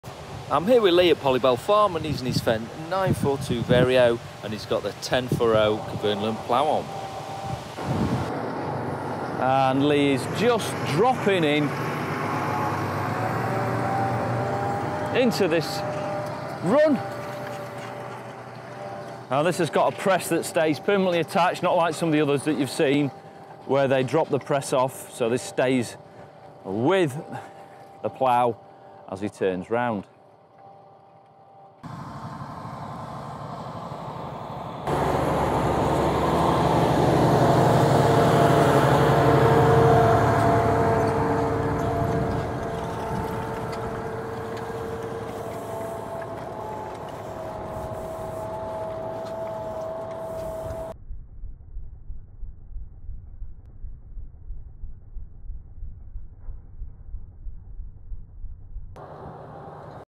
Fendt 942 Vario with a BIG plough
Here is Lee at Pollybell farm working in the field with the Fendt 942 vario and Kverneland 10 furrow variomat plough